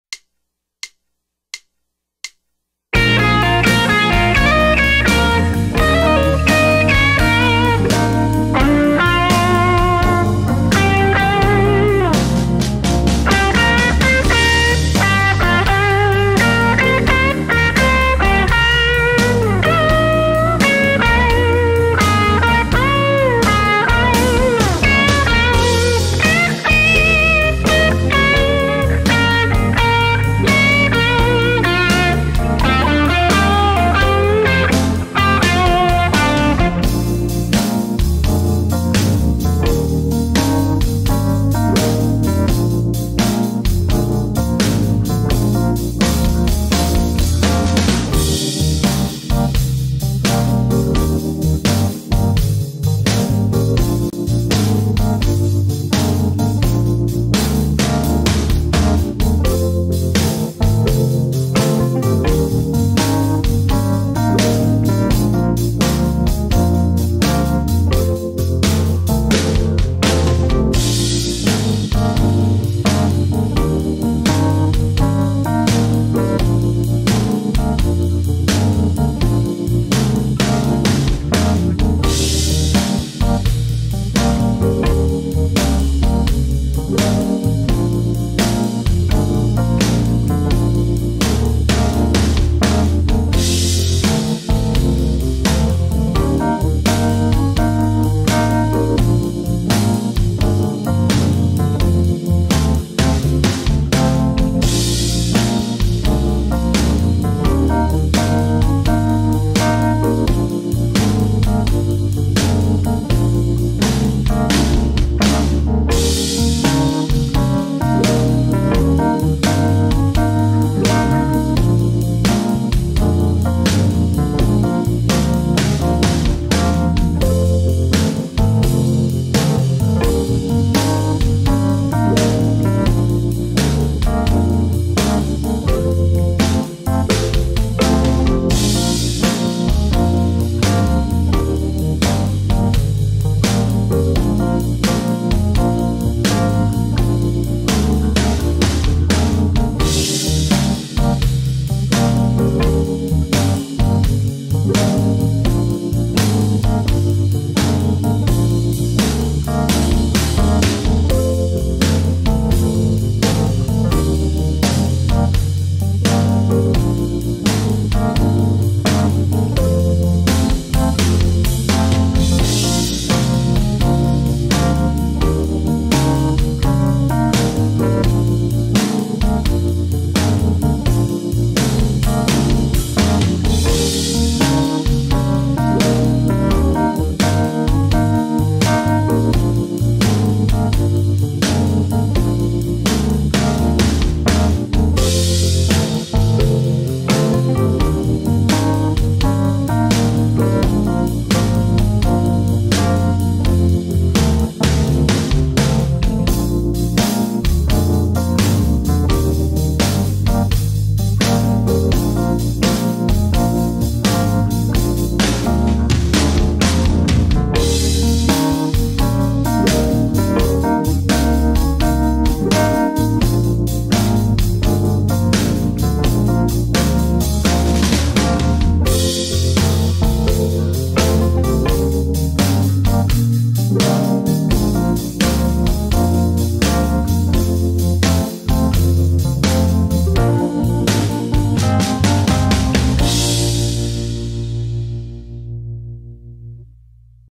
Vintage e-Piano Blues 85 Bpm in Bb major 12/8 Backing Track Jam W/ sample Licks
Twelve Bar Vintage e-Piano Blues 85 Bpm in Bb major 12/8 Backing Track Jam W/ sample Licks for Free Improvisation! Love Song,.
with background Vintage electric Piano part, over which a singer or soloist could play. Also includes electric bass, Piano, (Solo) e-Guitars and  Drum, ,.

Thanks